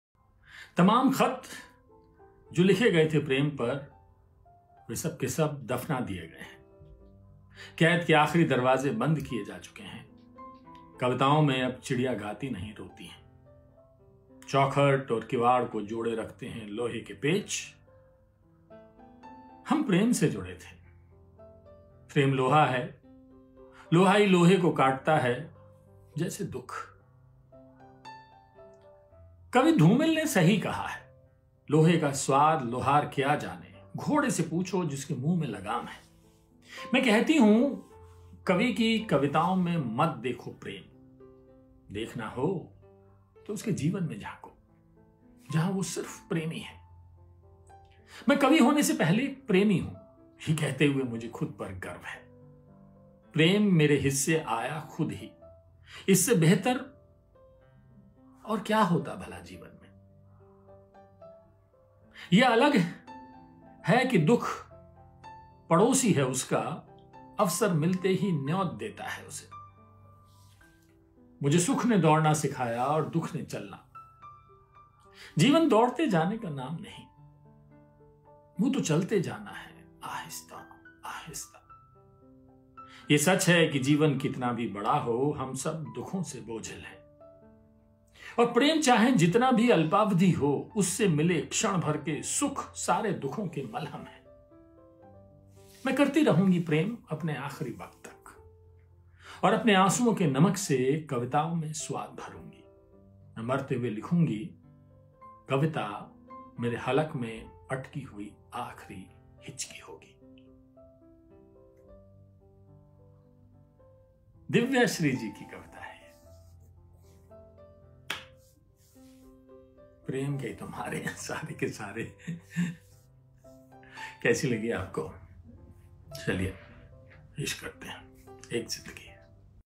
Prem Kavita | मैं कवि होने से पहले एक प्रेमी हूं | Divya Shree | Sanjeev Paliwal | Sahitya Tak
तमाम खत जो लिखे गए थे प्रेम पर
वे सबके सब दफना दिए गए हैं
कैद के आखिरी दरवाज़े बंद किए जा चुके हैं
कविताओं में अब चिड़िया गाती नहीं, रोती हैं

चौखट और किवाड़ को जोड़े रखते हैं लोहे के पेंच
हम प्रेम से जुड़े थे
प्रेम लोहा है
लोहा ही लोहे को काटता है
जैसे दुःख

कवि धूमिल ने सही कहा है
लोहे का स्वाद लोहार क्या जाने
घोड़े से पूछो
जिसके मुंह लगाम लगा है
मैं कहती हूं कवि की कविताओं में मत देखो प्रेम
देखना हो तो उसके जीवन में झांको
जहां वह सिर्फ प्रेमी है

मैं कवि होने से पहले एक प्रेमी हूं
यह कहते हुए मुझे खुद पर गर्व है
प्रेम मेरे हिस्से आया खुद ही
इससे बेहतर और क्या होता भला जीवन में
यह अलग है कि दुःख पड़ोसी है उसका
अवसर मिलते ही न्योत देता है उसे

मुझे सुख ने दौड़ना सिखाया
और दुःख ने चलना
जीवन दौड़ते जाने का नाम नहीं
वह तो चलते जाना है आहिस्ते आहिस्ते

यह सच है कि जीवन कितना भी बड़ा हो
हम सब दुखों से बोझिल हैं
और प्रेम चाहे जितना भी अल्पावधि हो
उससे मिले क्षणभर के सुख
सारे दुखों के मलहम हैं

मैं करती रहूंगी प्रेम अपने आखिरी वक्त तक
और अपने आंसुओं के नमक से
कविताओं में स्वाद भरूंगी
मैं मरते हुए लिखूंगी
कविता मेरे हल्क़ में अटकी हुई आखिरी हिचकी होगी... दिव्या श्री की शानदार कविता सुनें वरिष्ठ पत्रकार एवं लेखक संजीव पालीवाल से.